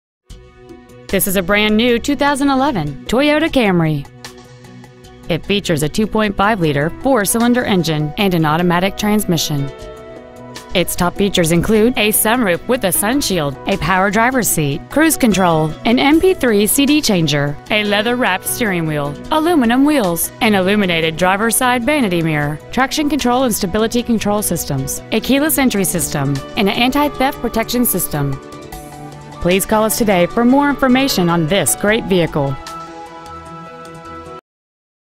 2011 Toyota Camry KY
We've been honored to serve the  KY area , we promise that your experience at our dealership will exceed your expectations !

 Year : 2011

 Make : Toyota

 Model : Camry

 Engine : 2.5L 4-Cylinder

 Trans . : automatic

 Exterior : Magnetic Gray Metallic

 Miles : 10

 18 Taft Hwy

 2011 Toyota Camry  KY